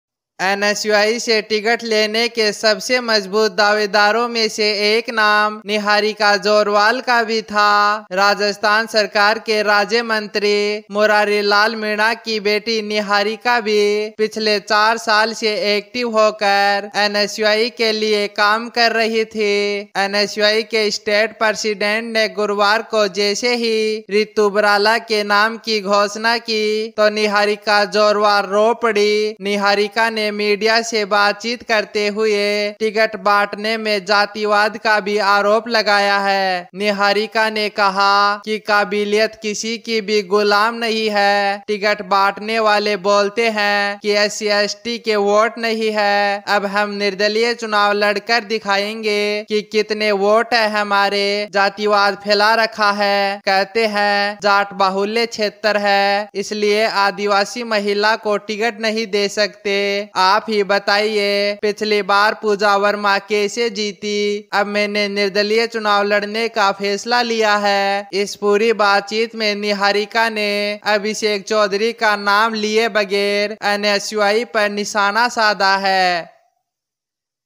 NSUI से टिकट कटने के बाद बोली निहारिका जोरवाल | Niharika Jorwal Rajasthan University
राजस्थान सरकार के राज्य मंत्री मुरारी लाल मीणा की बेटी निहारिका जोरवाल का राजस्थान विश्वविद्यालय से nsui का टिकट कट गया जिसके बाद निहारिका जोरवाल मीडिया के सामने रो पड़ी।
Niharika Jorwal, daughter of Minister of State in the Government of Rajasthan, Murari Lal Meena, was cut from Rajasthan University for NSUI, after which Niharika Jorwal wept in front of the media.